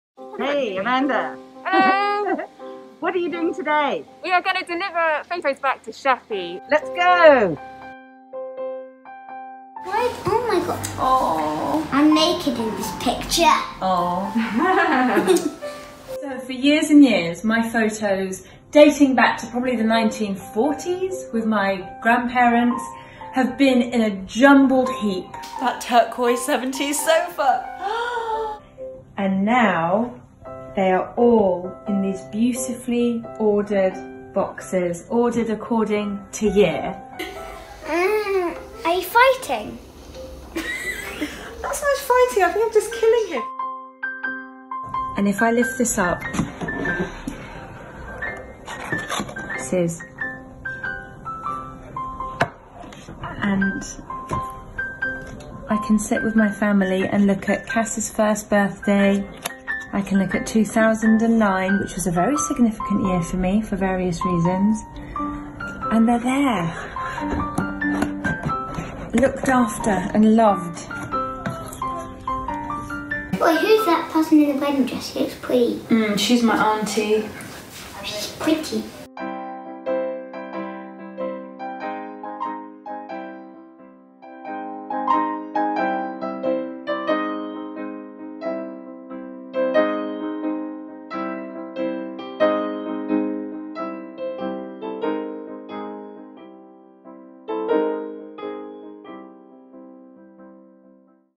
Photo organising for Shappi - the delivery
Shappi Khorsandi had a photo mess that needed a lot of photo organising.  Here is the happy result of all that hard work.